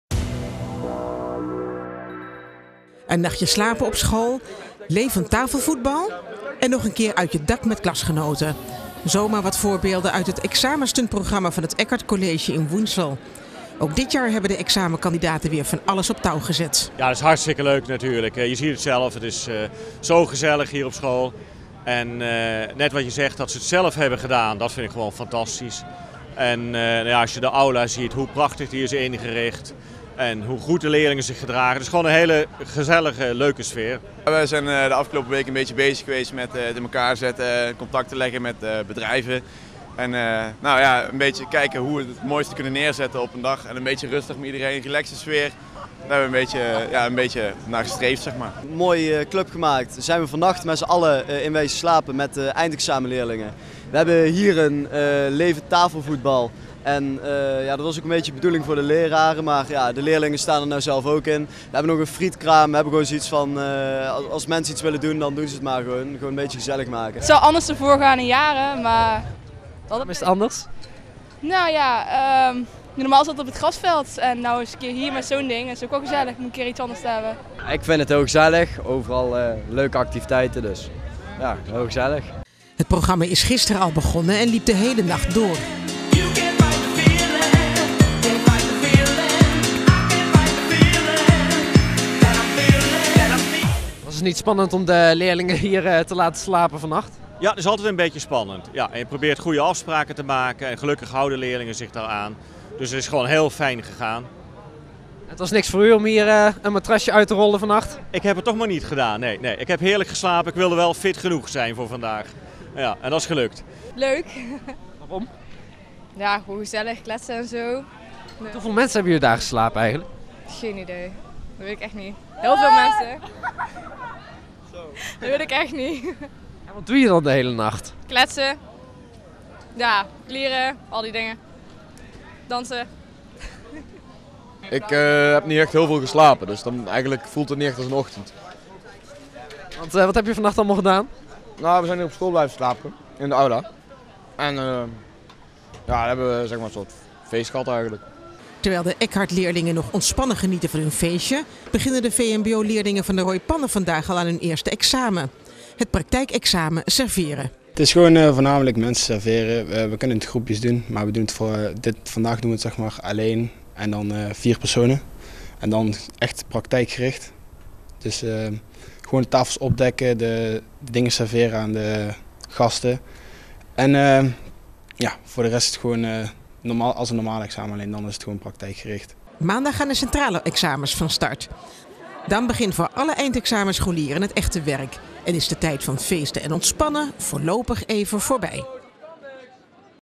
Eerste eindexamen gestart
Bij de Rooi Pannen was het vanmiddag er vanochtend al een eindexamen ingepland. Maandag gaan ook de examens op de HAVO en VWO van start.